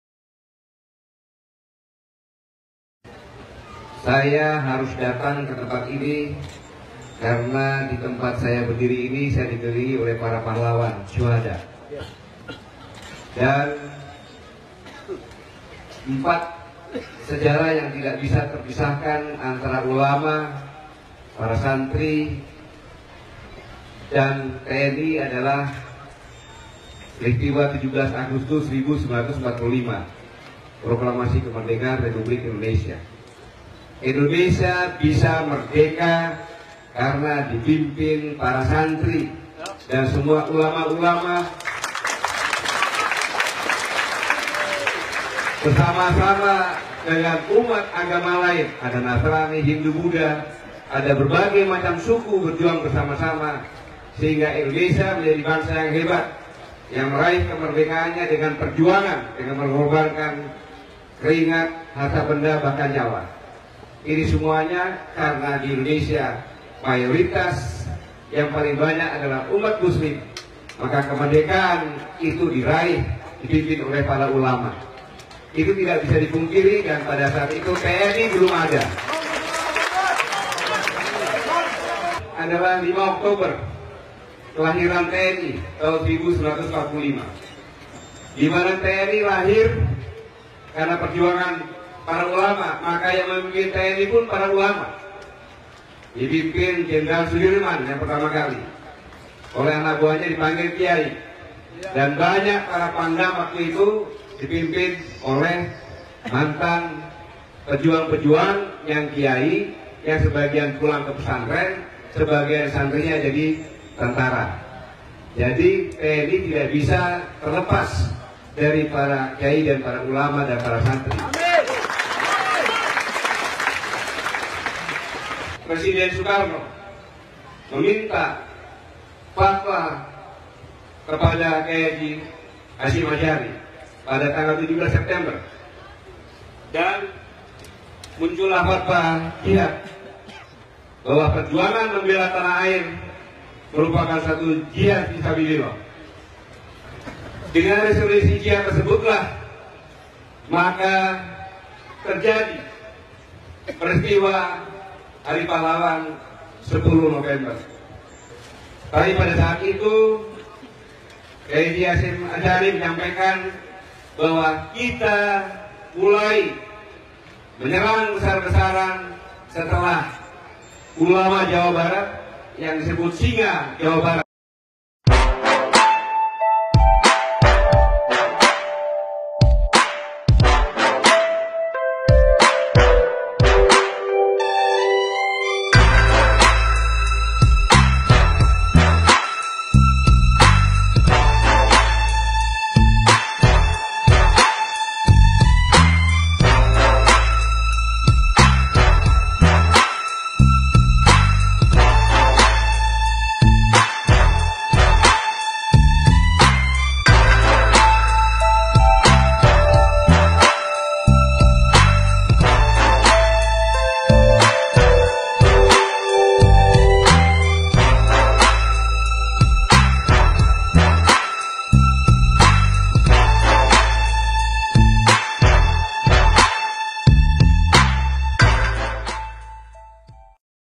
Panglima TNI di pondok buntet cirebon & kisah pilot TNI AU keturunan tionghoa
Panglima TNI jenderal gatot murmantiyo menyampaikan pentingnya peran ulama dalam menjaga NKRI,